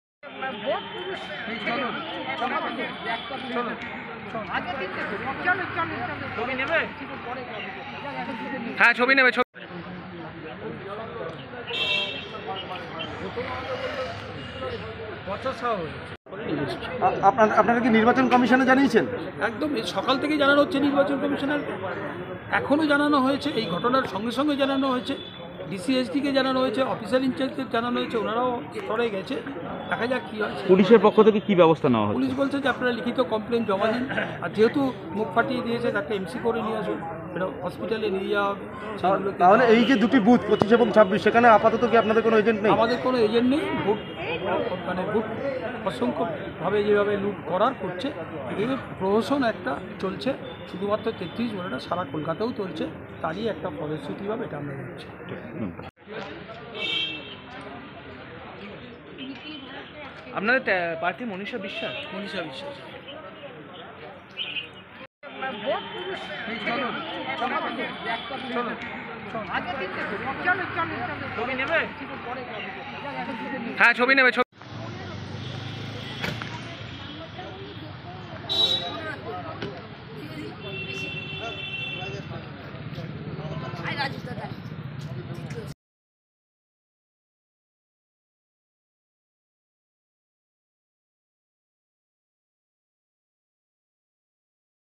এজেন্টকে মারধরের অভিযোগ থানা ঘিরে বিক্ষোভ বামেদের
রক্তাক্ত বাম এজেন্ট, ঘটনা ঘিরে বেলেঘাটা থানার সামনে সিপিএমের বিক্ষোভ, দায়ের করা হল লিখিত অভিযোগ